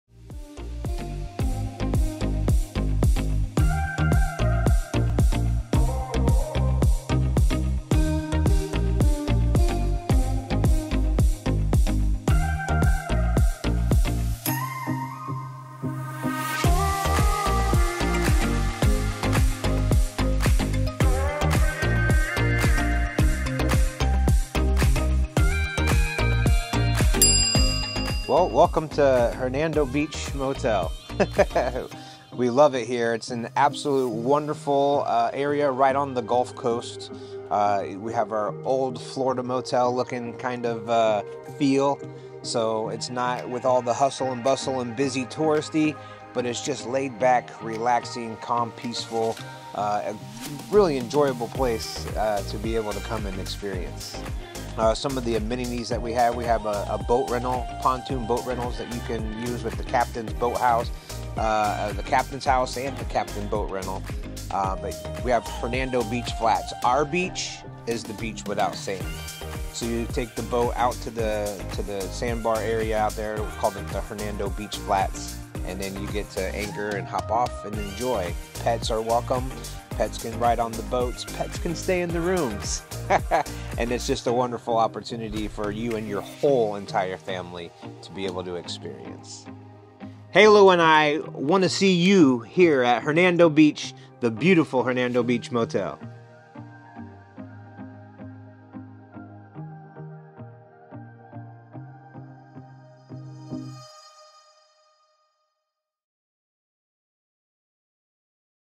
Halo's Hangouts on Florida's Adventure Coast - Hernando Beach Motel (2022)
​@halothegoldenretriever Tourism Dog had a great time discovering the Hernando Beach Motel. A quaint and charming motel right on the waterways of the Gulf of Mexico.

Learn more about Florida’s Adventure Coast, Brooksville-Weeki Wachee, by visiting our